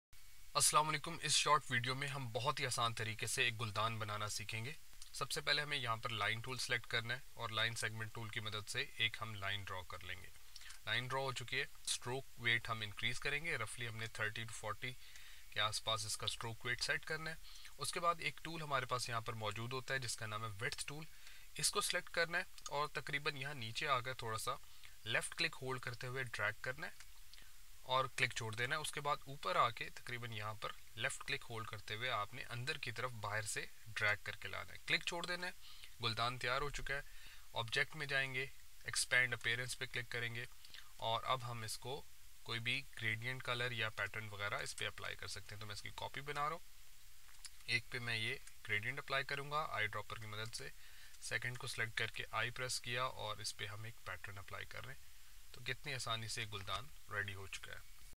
How to make a Vase in Illustrator 2022 | RightPixels
In this video, you will learn how to make a flat, simple but beautiful vase in Adobe Illustrator with the help of width tool. This Trick be a time savior. Its not a 3D vase but a 2D flat Vase.